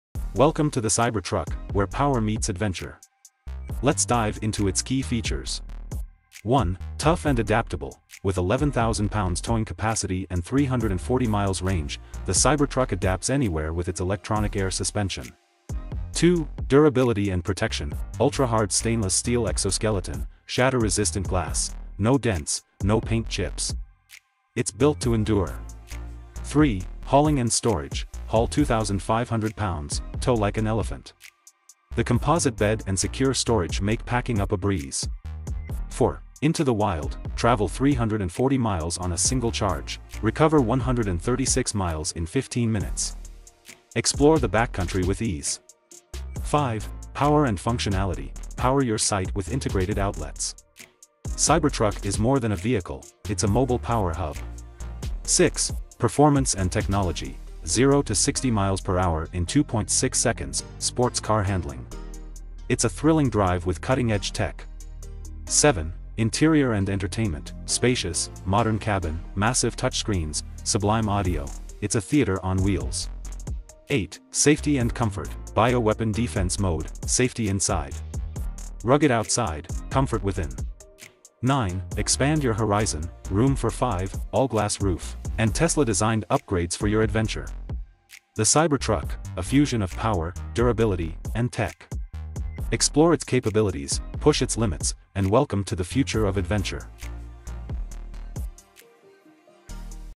Cybertruck: The Cyberbeast 2024 🚚⚡👽
Dive into a world where power meets adventure! 🌐 Join us as we explore the groundbreaking features of the Tesla Cybertruck—tough, adaptable, and loaded with cutting-edge tech. From its impressive towing capacity to the thrill of Beast Mode acceleration, the Cybertruck is more than a vehicle; it's a statement. 🚗💨 Charge up your excitement for the future of driving with us!